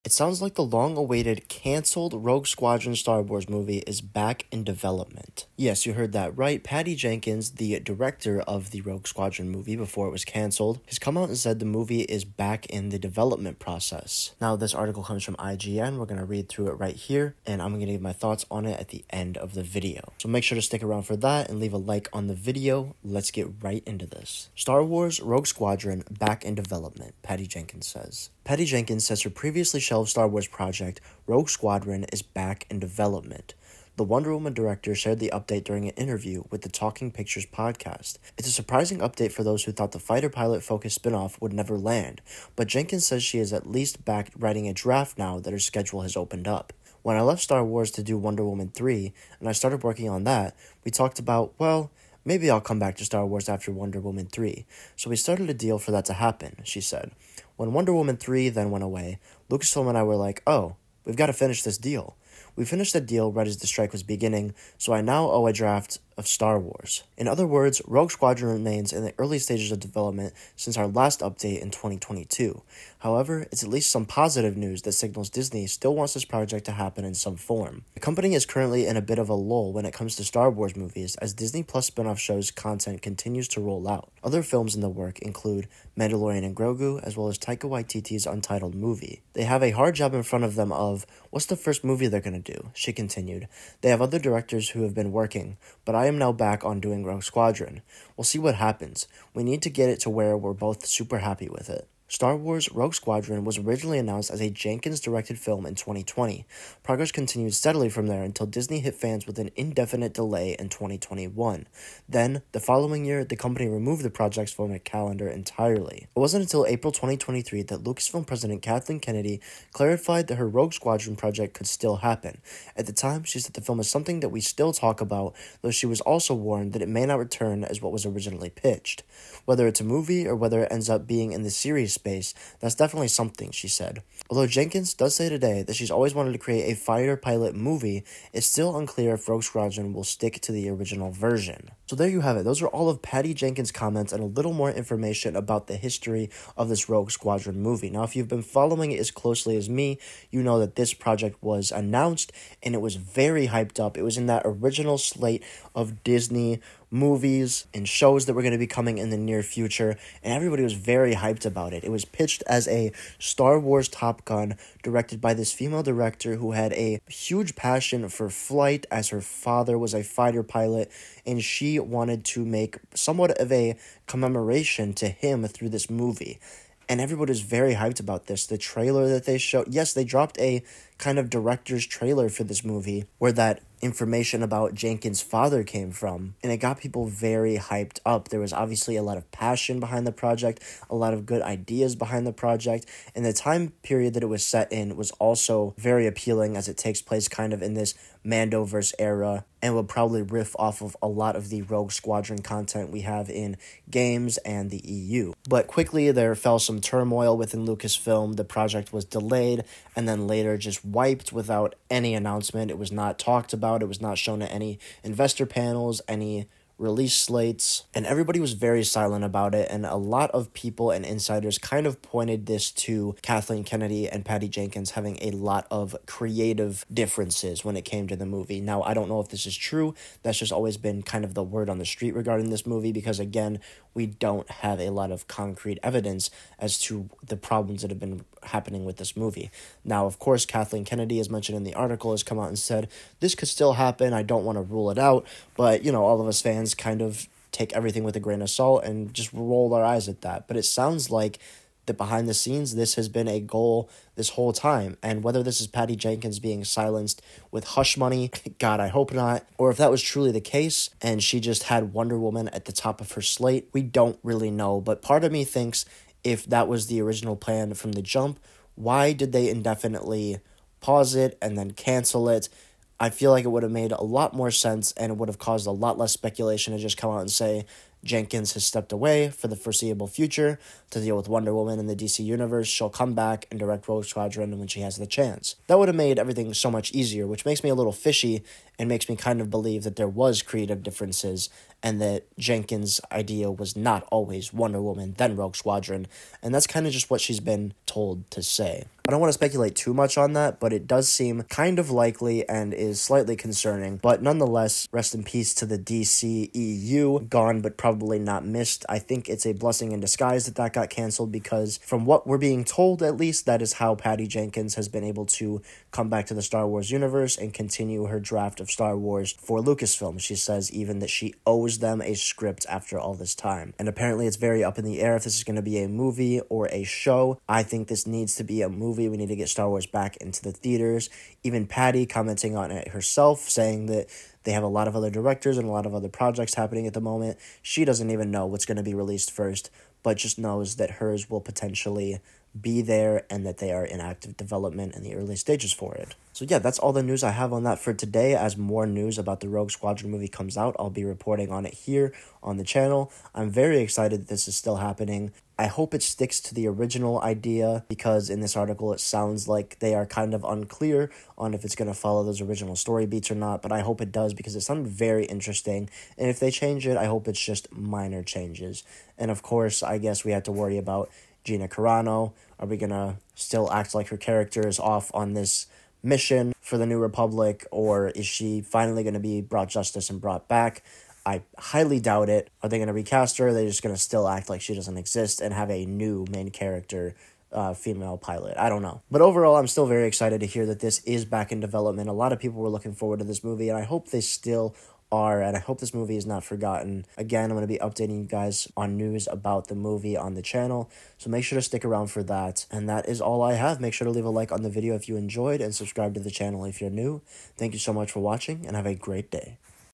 The Rogue Squadron Movie is Still Happening
In today's video I will be covering the recent news that Patty Jenkins will indeed be returning to write and direct the cancelled Star Wars" Rogue Squadron movie.

Welcome to the Shack Family!